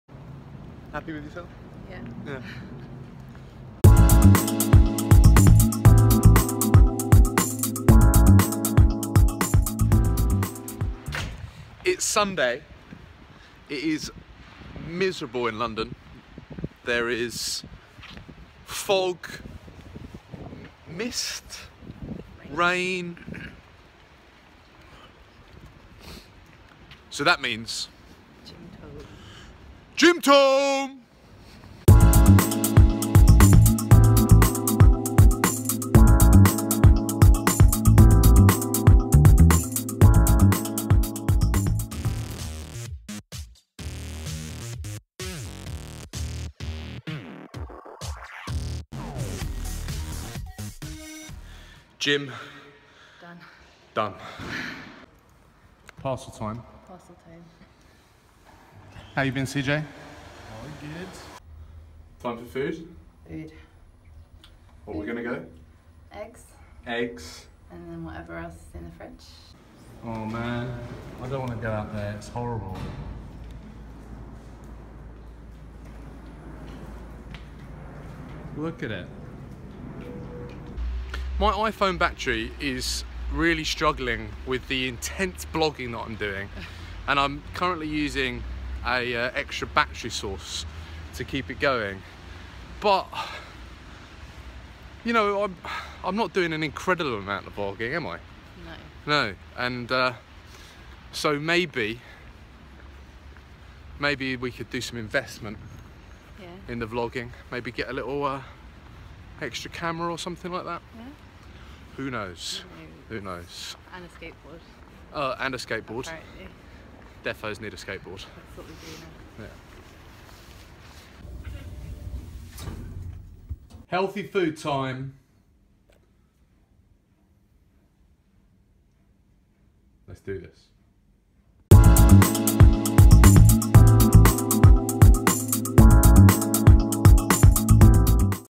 Sunday means Gym Tome!! JODVLOG #010
London is wet, cold and rainy.

So for Charlie and me that means healthy food and GYM TOME!!

My FAVOURITE shoes: Vivobarefoot
My FAVOURITE sunglasses: Breo
My FAVOURITE laptop: Apple
My FAVOURITE watch: Casio
My FAVOURITE notebook: Moleskin